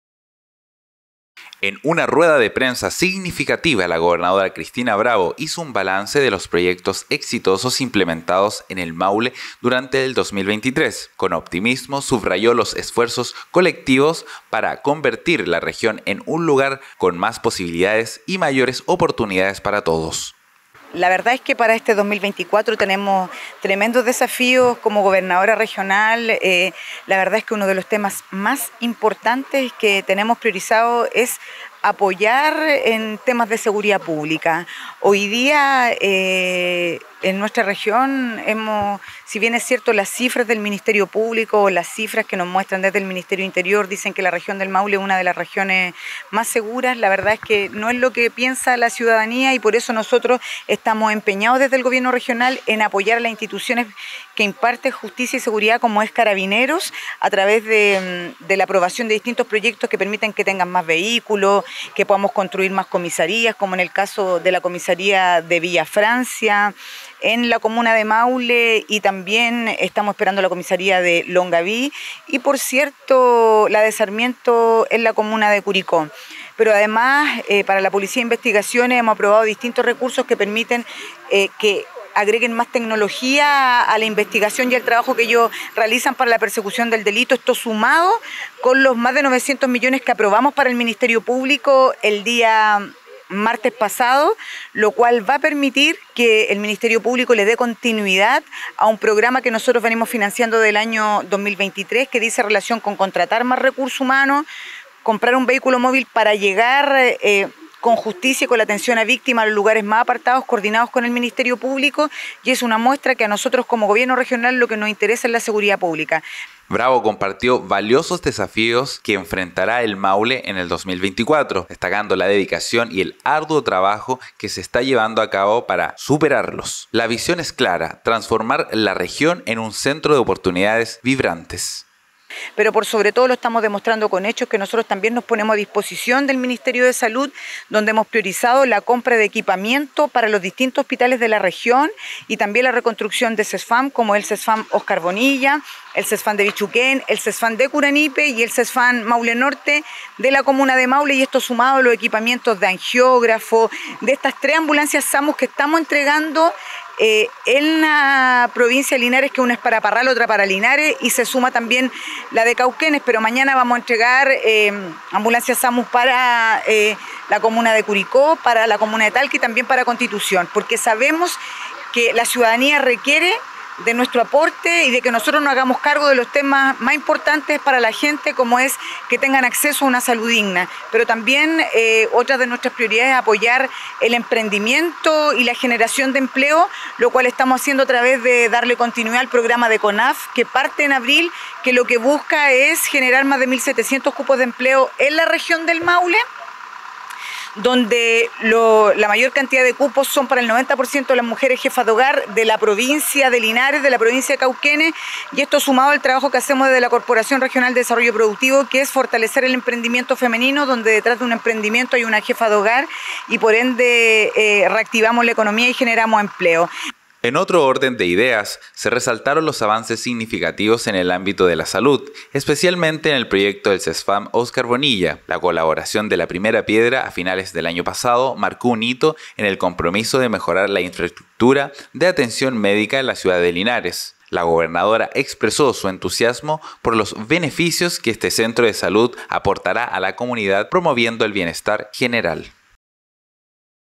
Gobernadora Bravo: logros y desafíos 2024 | Lunes 29.01.24
En una rueda de prensa significativa, la gobernadora Cristina Bravo hizo un balance detallado de los proyectos exitosos implementados en el Maule durante el 2003. Con optimismo, subrayó los esfuerzos colectivos para convertir la región en un lugar con más posibilidades y mayores oportunidades para todos.

Nota de prensa emitida el día lunes 29 de enero del 2024 en la edición central de nuestro noticiero Maule Informa